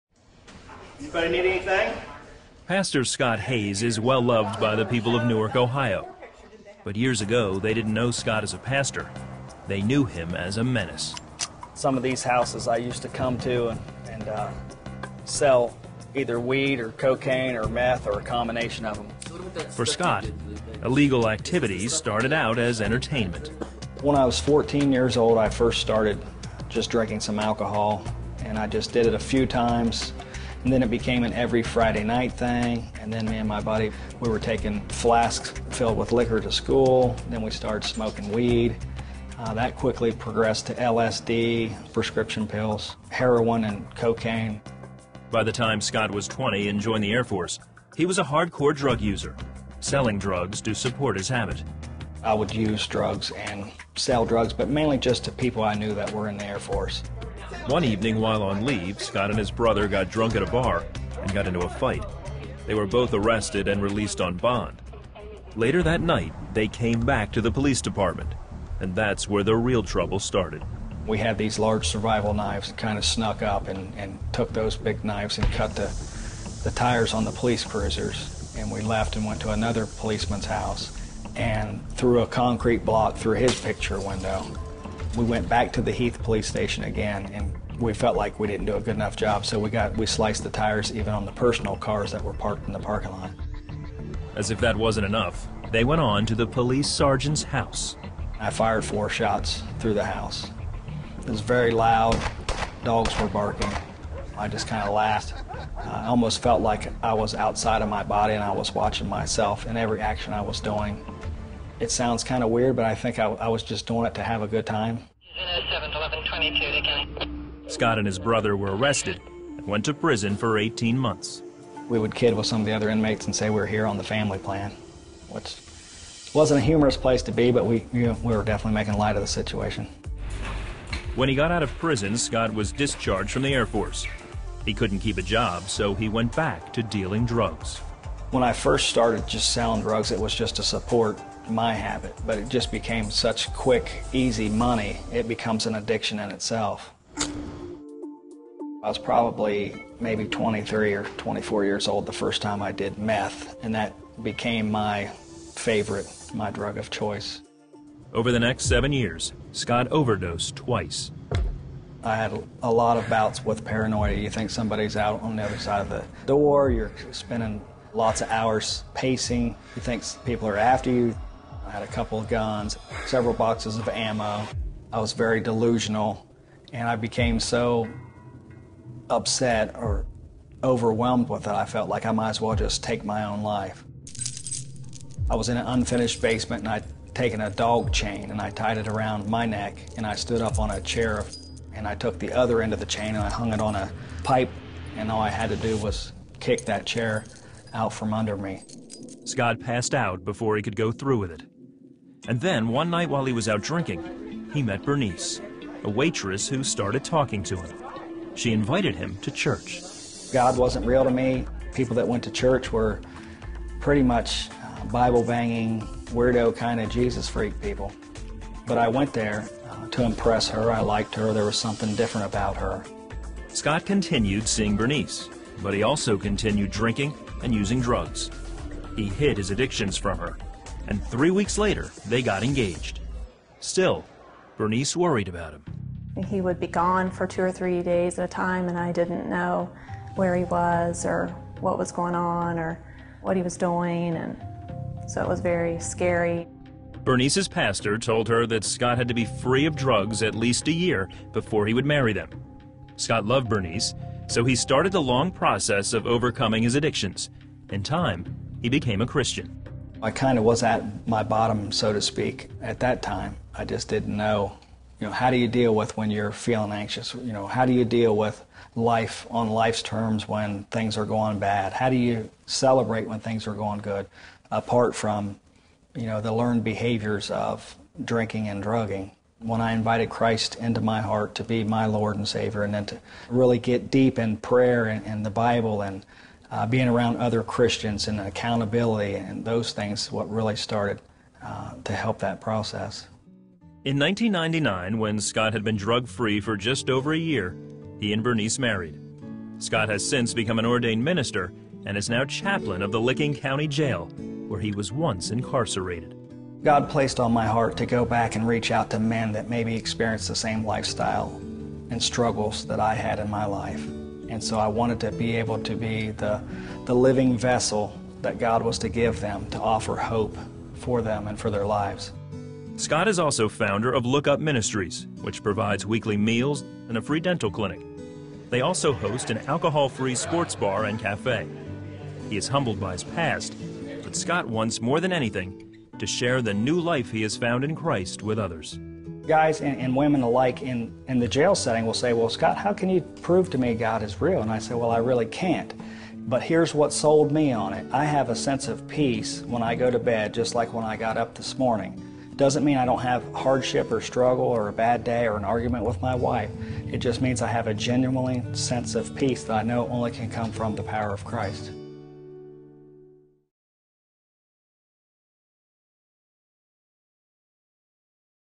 A Troublemaker Discovers Genuine Peace - CBN.com
Scott Hayes grew up a troublemaker. At 14, he began drinking and smoking pot. By adulthood, Scott was an alcoholic, drug dealer, and criminal... The Christian Broadcasting Network CBN